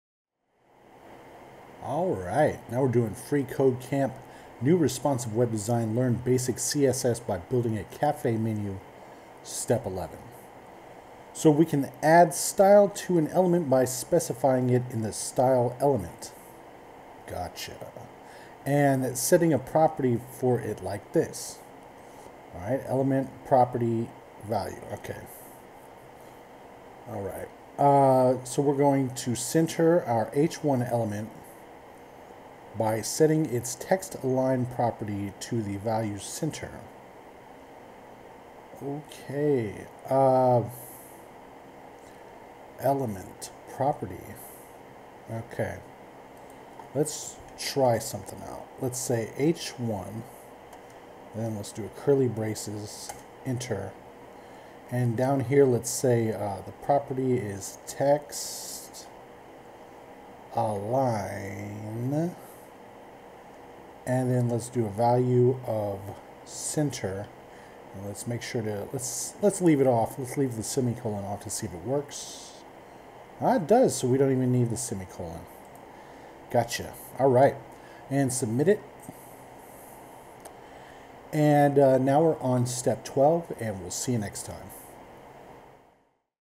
Learn Basic CSS by Building a Cafe Menu - Step 11
this one introduces setting a style property for an element